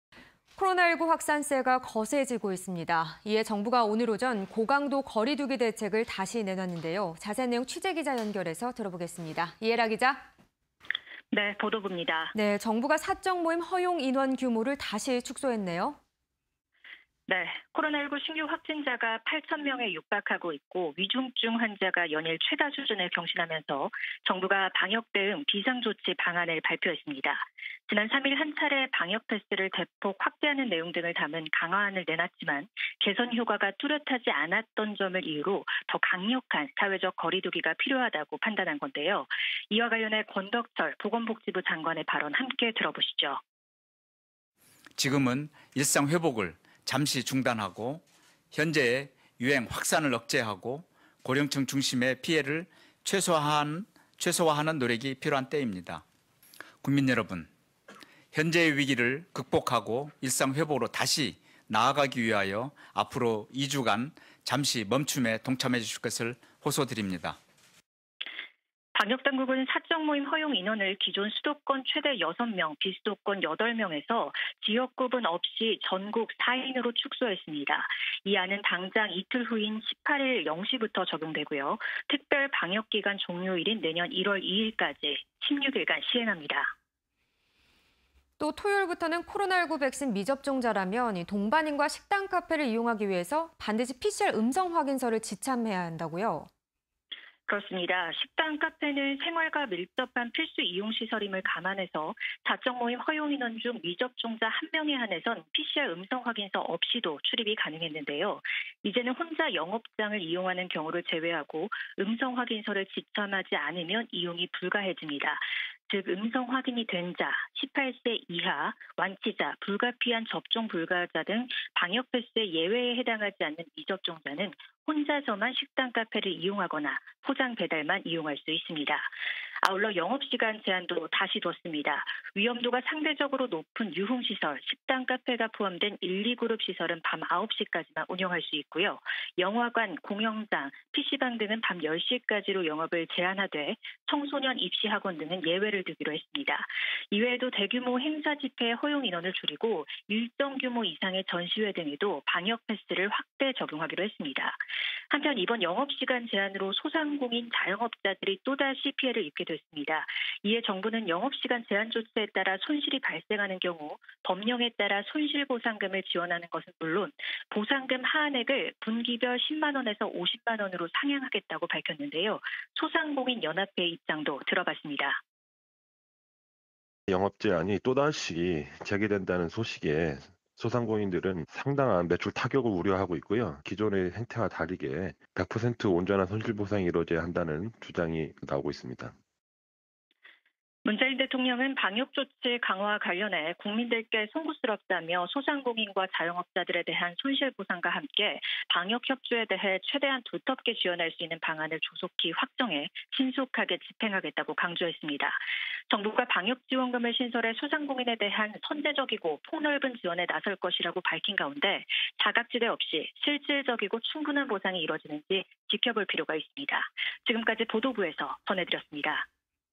모레부터 ‘인원·영업시간 제한’… 소상공인 ‘울상’ (20211216)
코로나19 확산세 심화… 방역 강화 2주만에 수위 높여
- 18일부터 "사적모임 4인까지"… 16일간 시행
- 미접종자 '혼밥'만 가능… 모임 참석시 음성확인서 지참해야
- 식당·카페·영화관 등 영업시간 9~10시로 제한
- 정부 "보상금 하한 상향·방역지원금 신설할 것"
- 文대통령 "국민께 송구… 최대한 두텁게 지원"